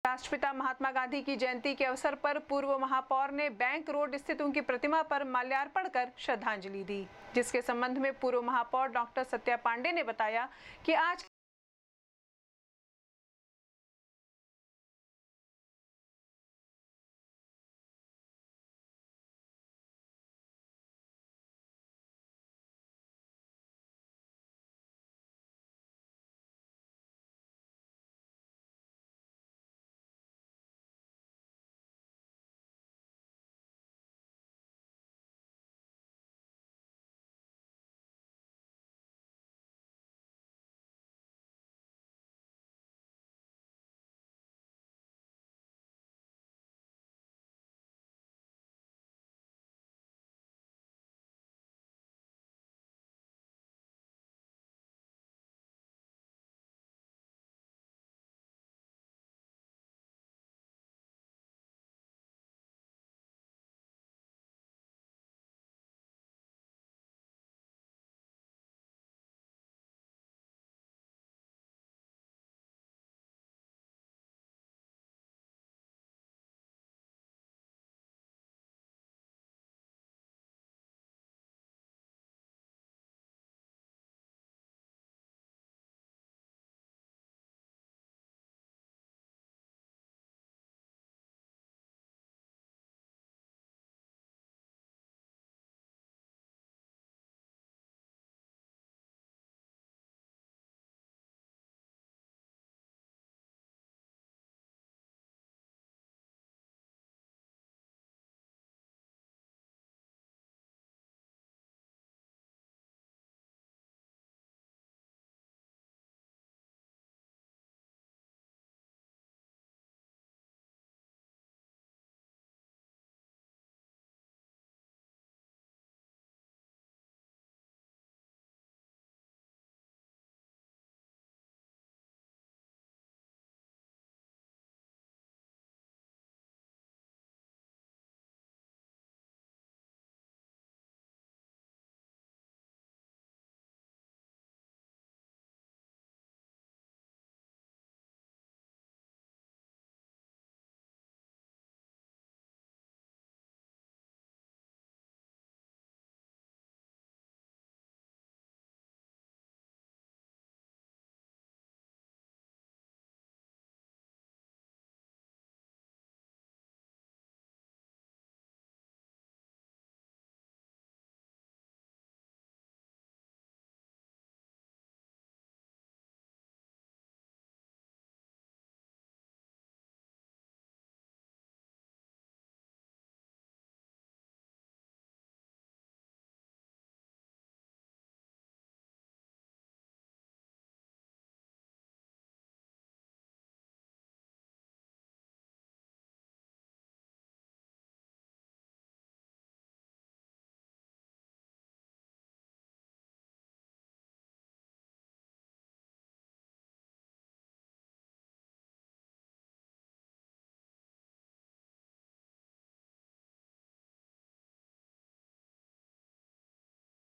राष्ट्रपिता महात्मा गांधी की जयंती के अवसर पर पूर्व महापौर ने बैंक रोड स्थित
राष्ट्रपिता महात्मा गांधी की जयंती के अवसर पर पूर्व महापौर ने बैंक रोड स्थित उनके प्रतिमा पर माल्यार्पण कर दी श्रद्धांजलि, उनके पद चिन्हों पर चलने का लिया संकल्प